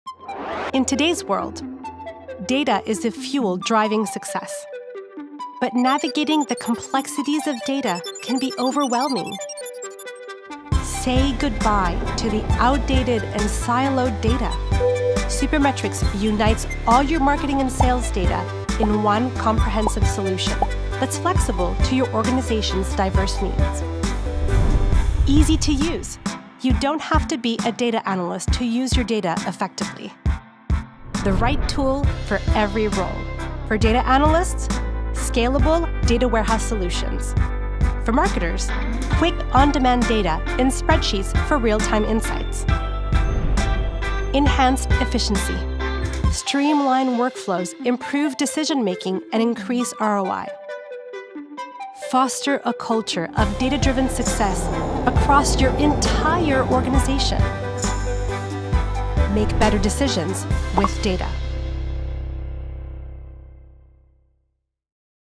Make better decisions. With data.
No organization or marketing team is quite the same.

Everyone’s operating across their own domains with their own set of data and tools.

👉 We have…. ad hoc questions, experiments, rapid deployment, and all the quick tasks frontline marketers do in their spreadsheets and dashboards.
👉 But we also have… historical data, advanced modeling projects, MMM, with data stored in one centralized location that the data team uses.

Whether you need to dive deep or dash fast with your data, Supermetrics allows you to do both… and scale with your growing data needs so you can make better decisions with data.